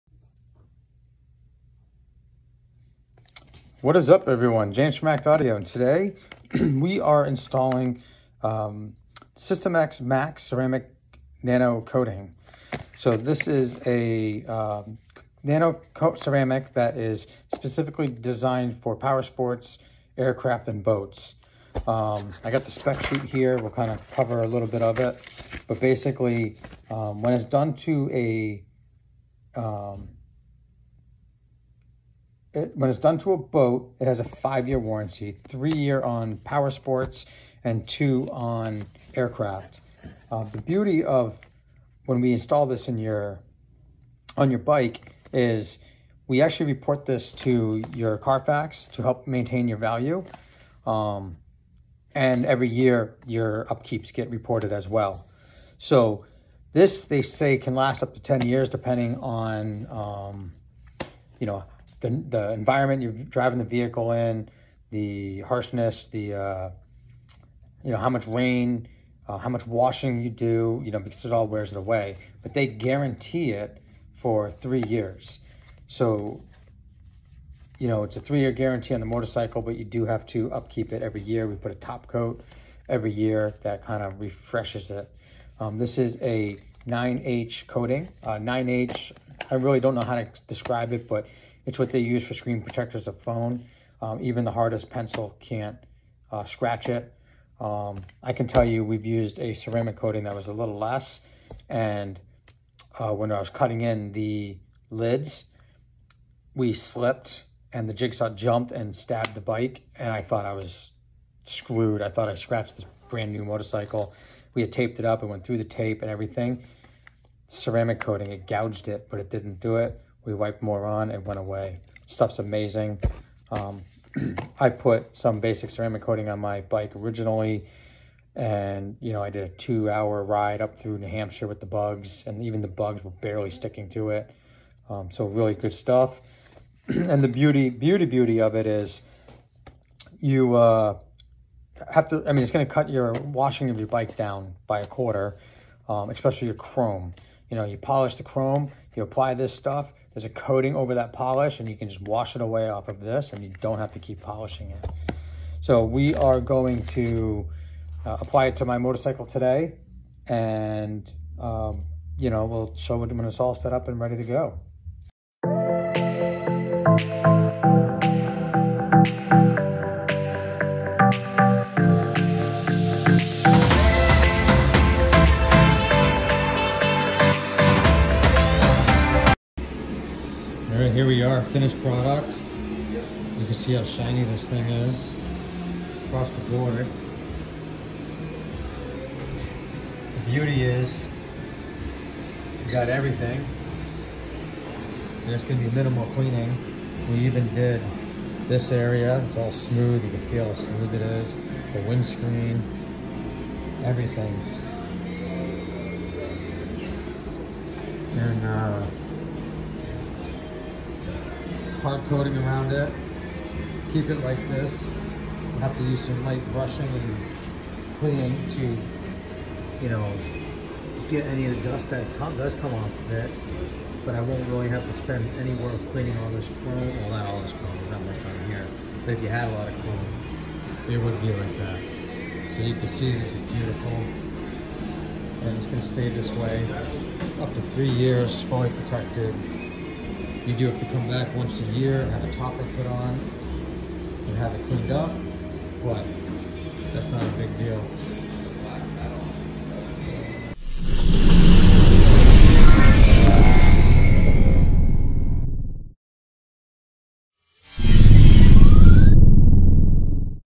Review of ceramic coating my motorcycle, the end is the product
This video is a review of system X max ceramic coating on my motorcycle. Watch until the end to see the results.